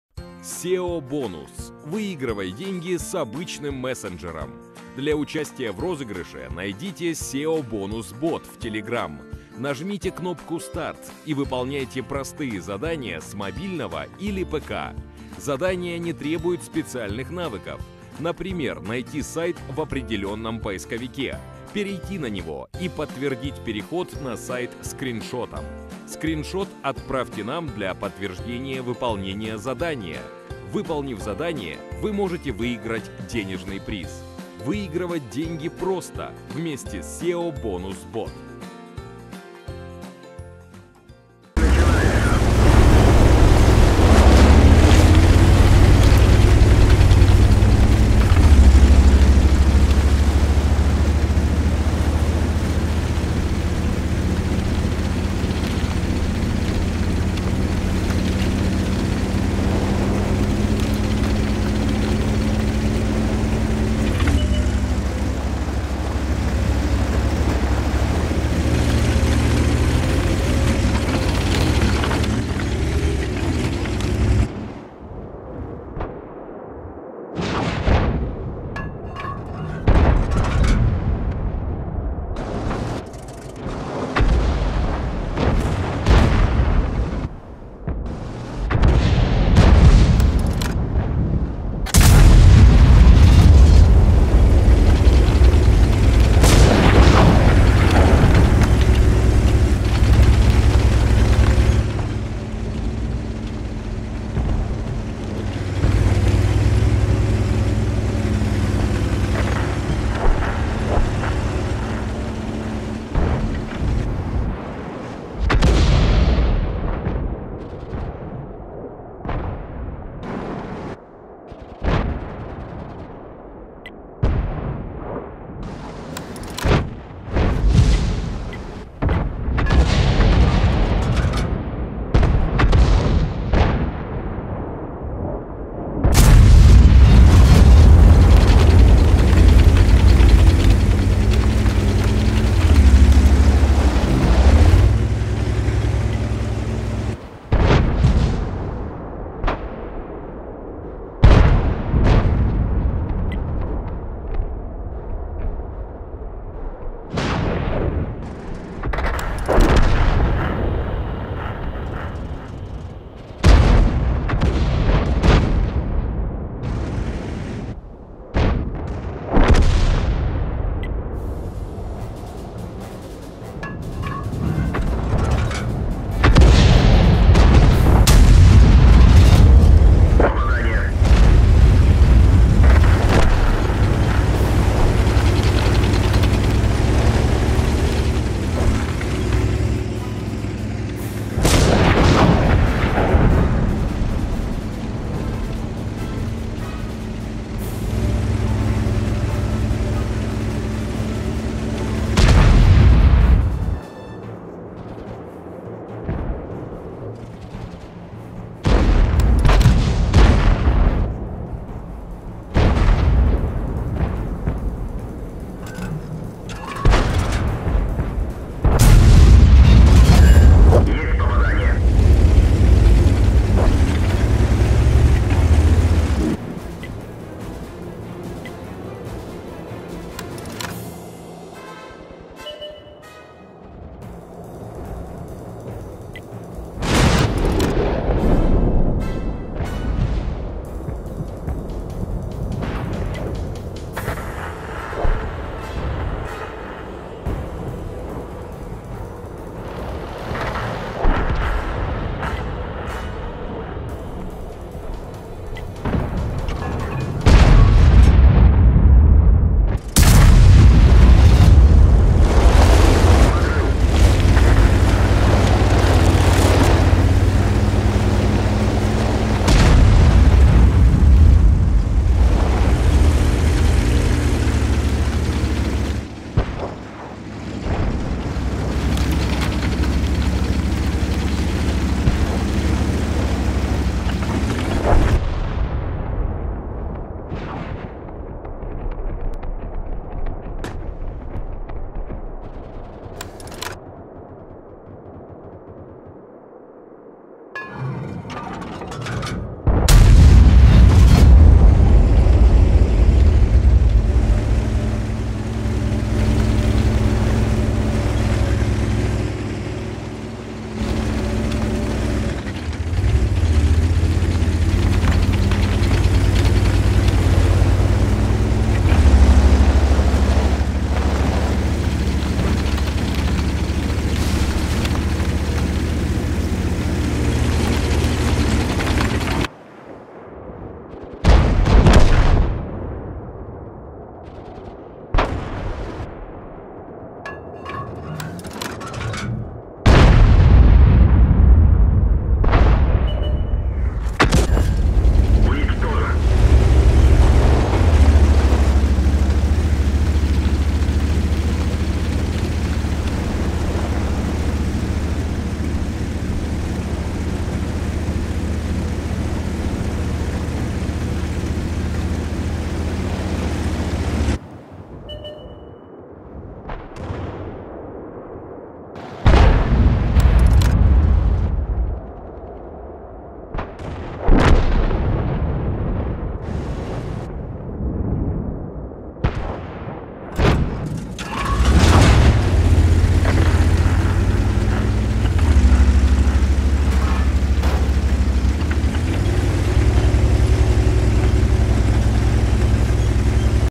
Медленная арта в Харькове
Автор реплея: Play-replay
Карта: Харьков
Танк: G.W. Tiger (P)
Кредитов: 29376
Фрагов за бой: 1
Урон за бой: 3059
Заблокировано урона: 1
Опыт за бой: 963
Описание: Иногда даже самая медленная арта может в городе.